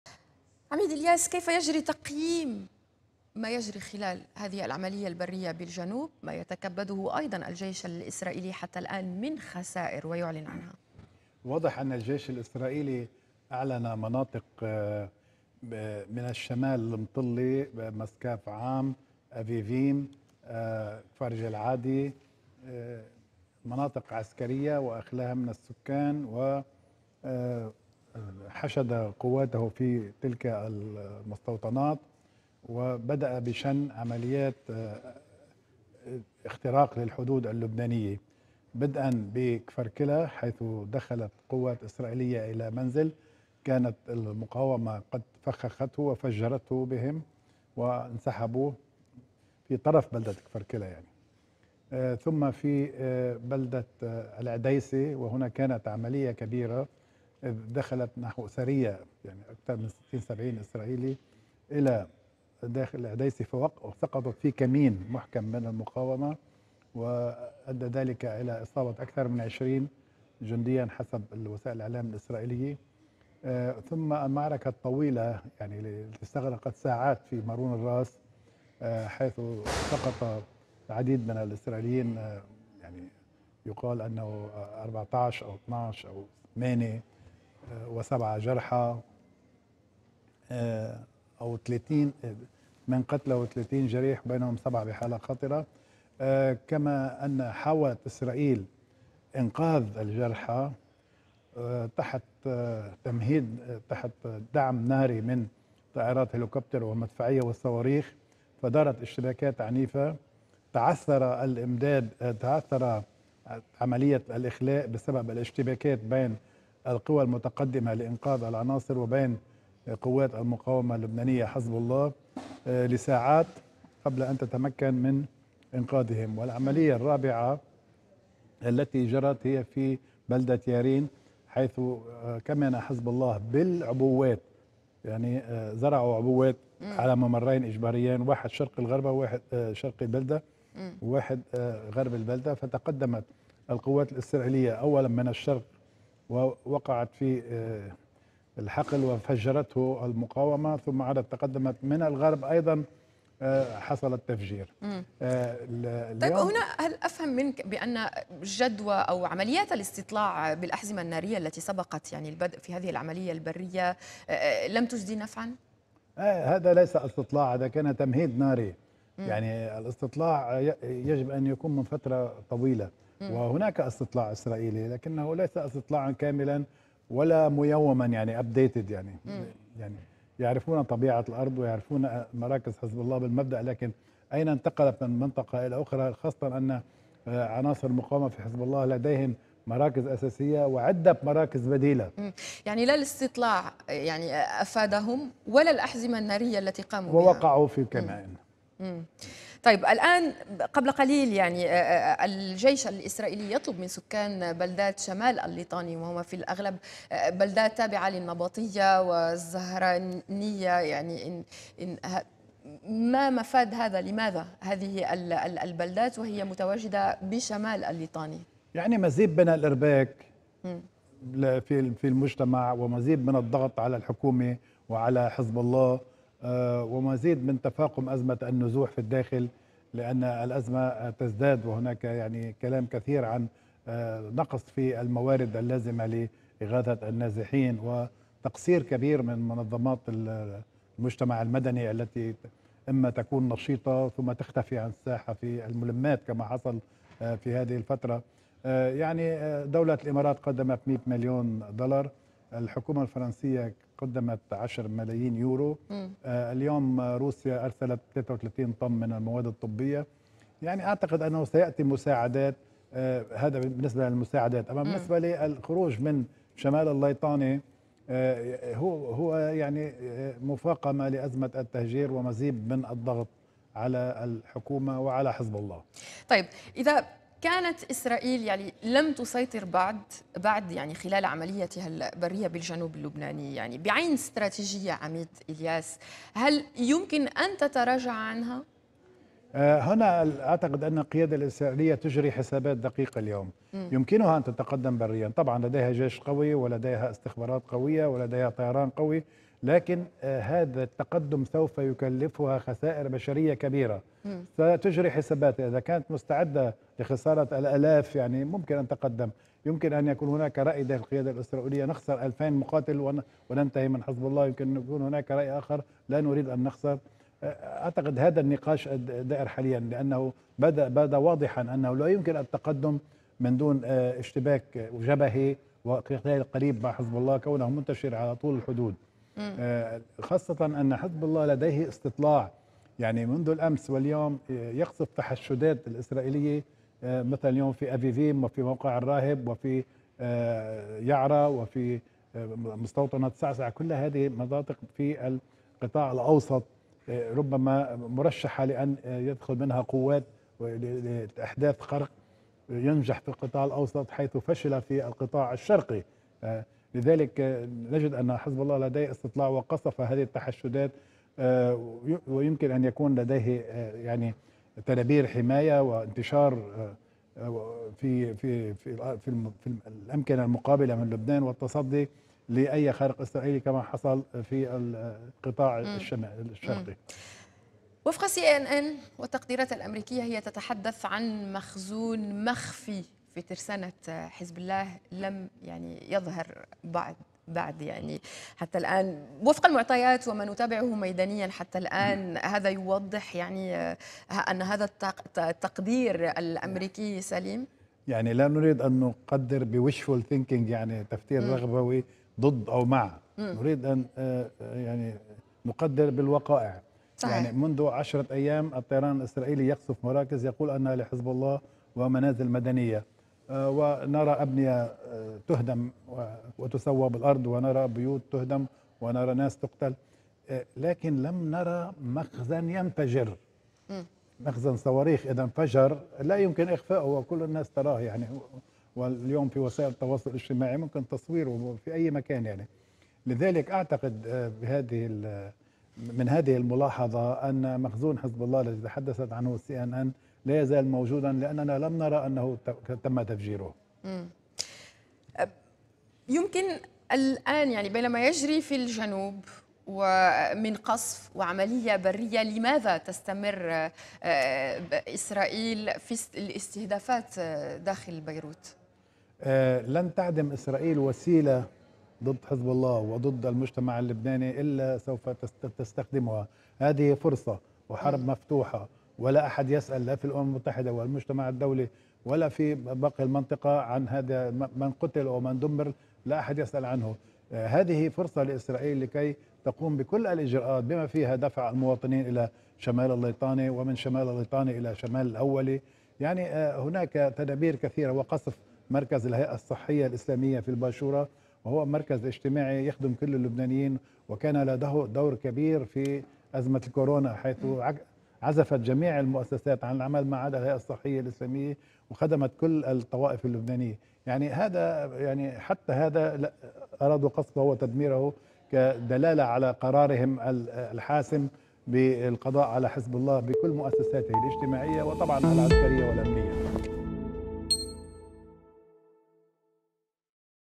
إلياس فرحات: التوغل الإسرائيلي في لبنان سيكلفها خسائر بشرية كبيرة
مستمرون في تغطيتنا من قناة الغد للعدوان الإسرائيلي على قطاع غزة والضفة ولبنان.. لمزيد من التفاصيل يتحدث العميد إلياس ‏فرحات المحلل العسكري والاستراتيجي‏ مع الإعلامية كوثر تنوري